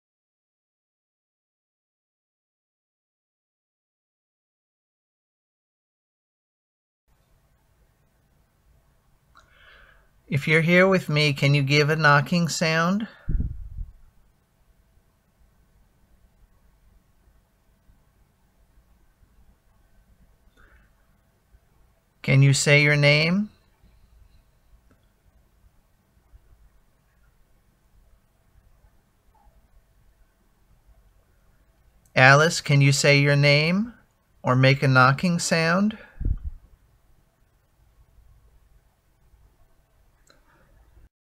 TRYING TO REACH THE SPIRIT OF MY WIVE'S GRANDMOTHER
Speaking with spirits using my psb7scanning radio that allows spirits to speak through the white noice static.